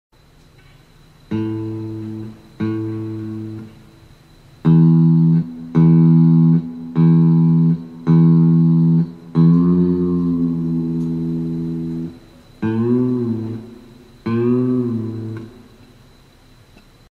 Setting Saddle into Bend Position
Showing how to enable string bending by setting the saddle to the end of zone 2.